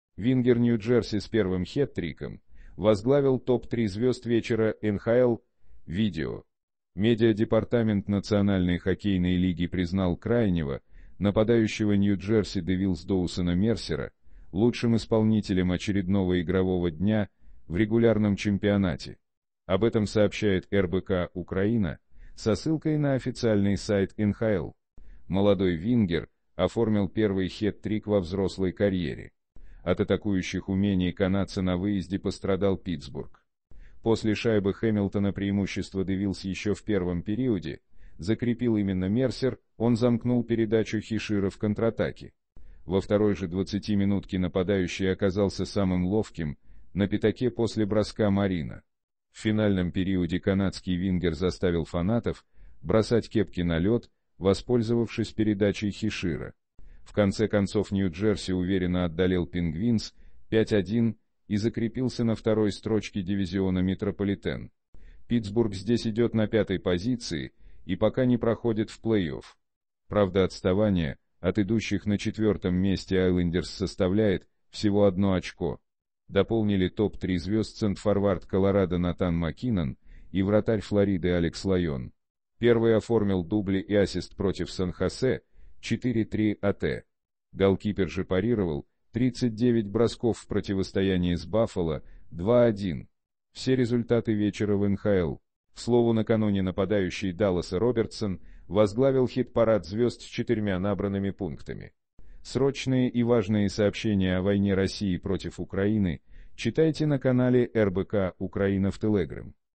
Вингер "Нью-Джерси" с первым хет-триком возглавил топ-3 звезд вечера НХЛ: видео.
НХЛ хоккей Медиа-департамент Национальной хоккейной лиги признал крайнего нападающего "Нью-Джерси Девилз" Доусона Мерсера лучшим исполнителем очередного игрового дня в регулярном чемпионате. Об этом сообщает РБК-Украина со ссылкой на официальный сайт НХЛ. Молодой вингер оформил первый хет-трик во взрослой карьере. От атакующих умений канадца на выезде пострадал "Питтсбург". После шайбы Хэмилтона преимущество "Девилз" еще в первом периоде закрепил именно Мерсер – он замкнул передачу Хишира в контратаке. Во вто